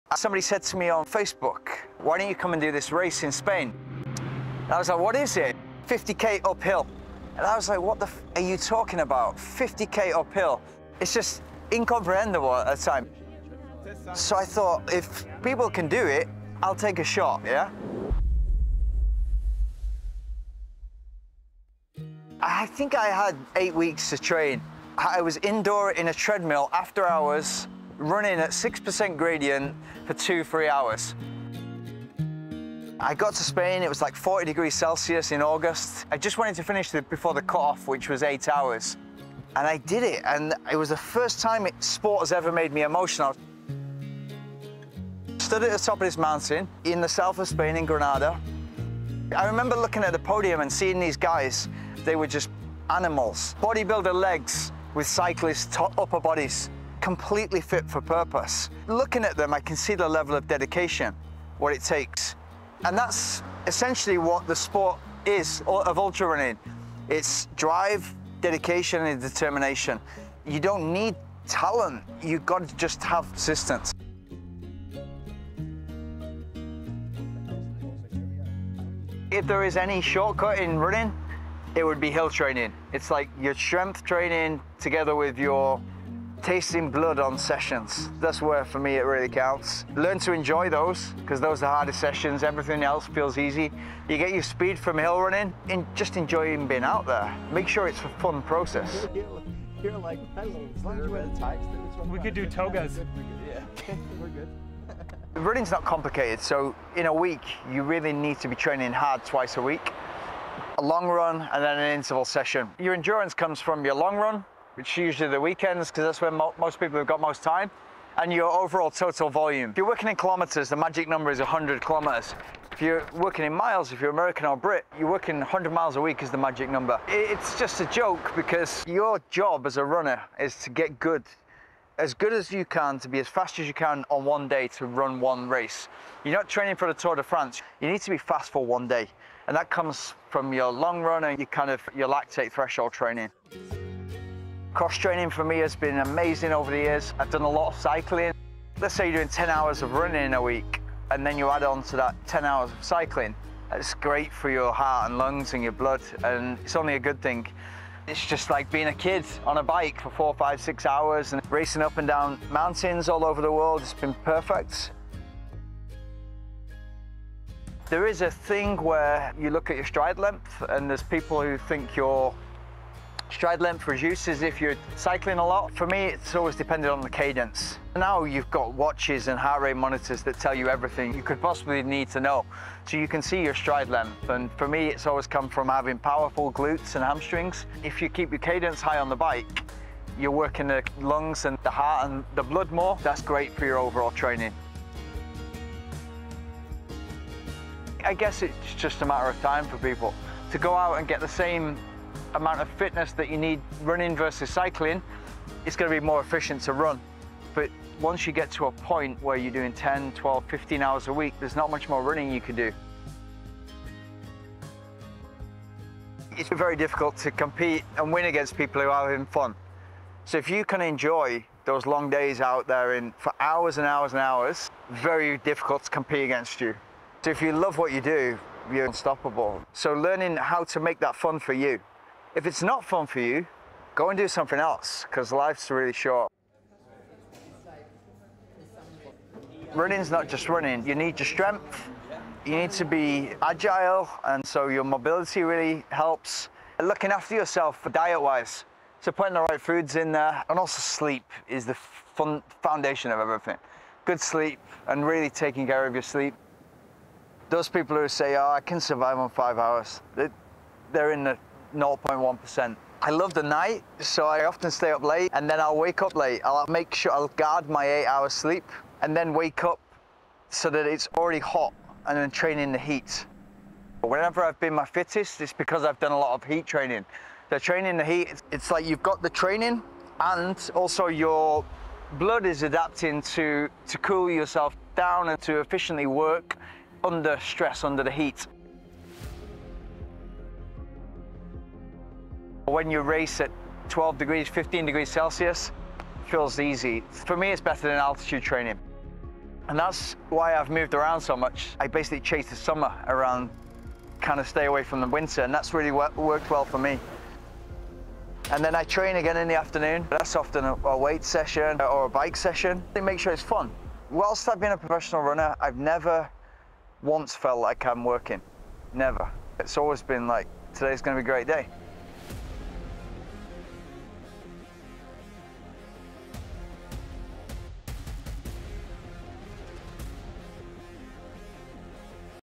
How to Run Faster: Trail Running Endurance
Ultra-distance runner Lee Grantham shares his pro tips on how to run faster and what it takes to become a better runner.

The Craft Elite Team:
Tommy Rivs, Arlen Glick, Tim Freriks, Tim Tollefson, Emkay Sullivan, Lee Grantham, Manuela Vilaseca, Alexander Holmblad, David Laney, Jacob Puzey, Tessa Chesser, Mimmi Kotka, Ida Nilsson.